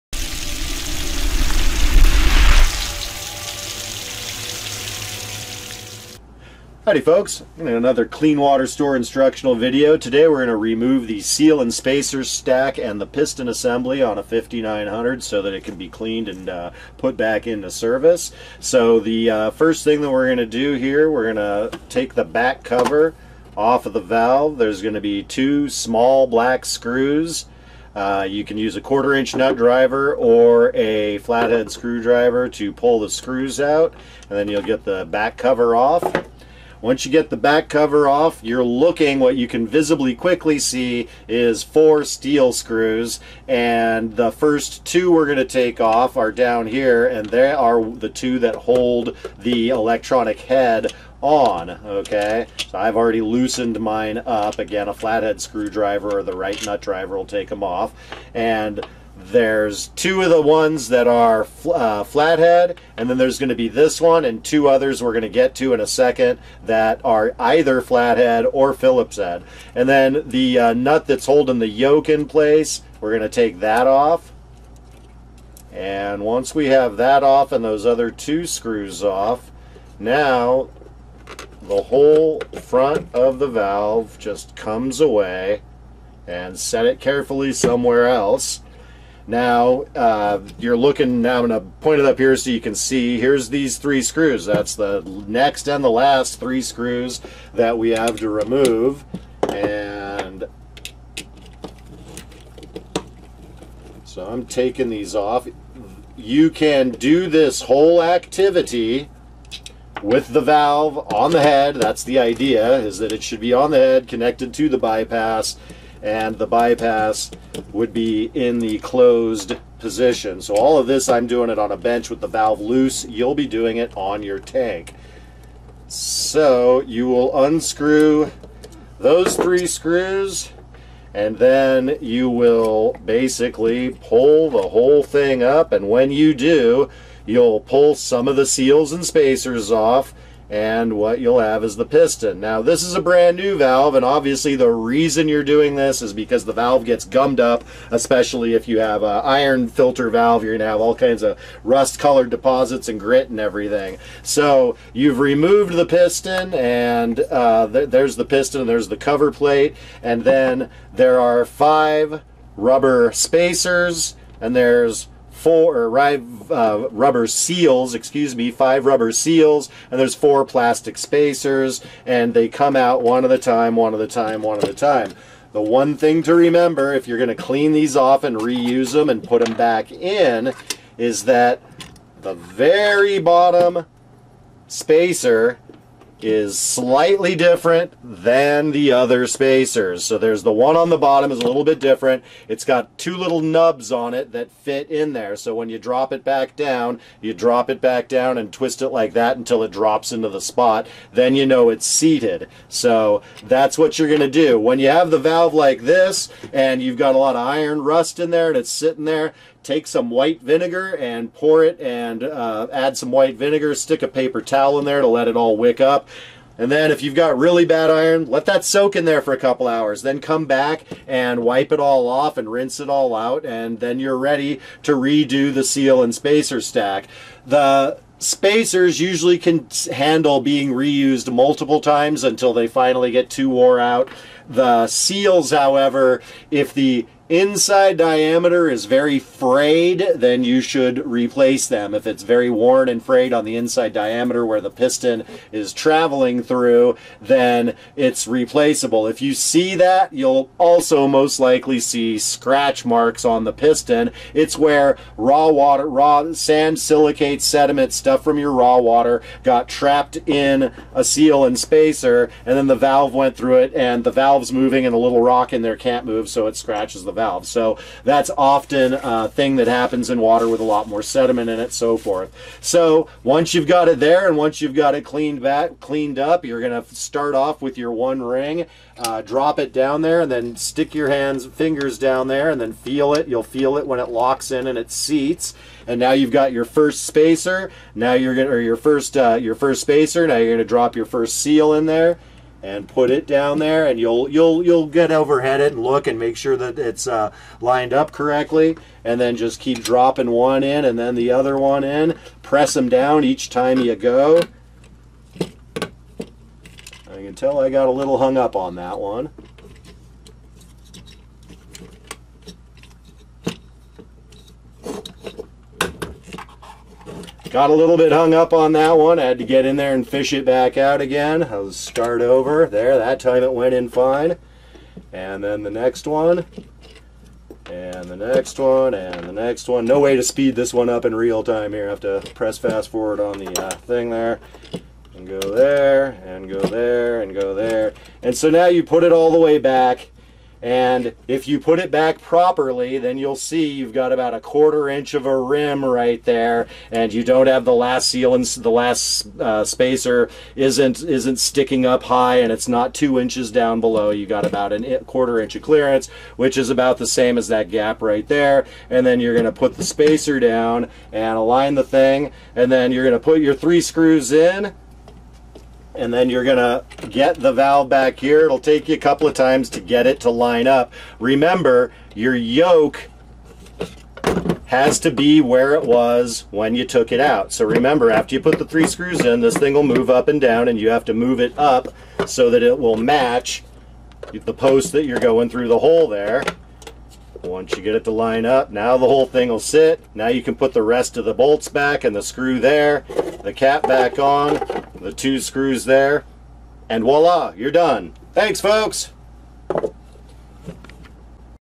5900-piston-seal-service
How to service the piston, seals and spacers on a 5900 control valve for iron treatment, and other well water filtration needs.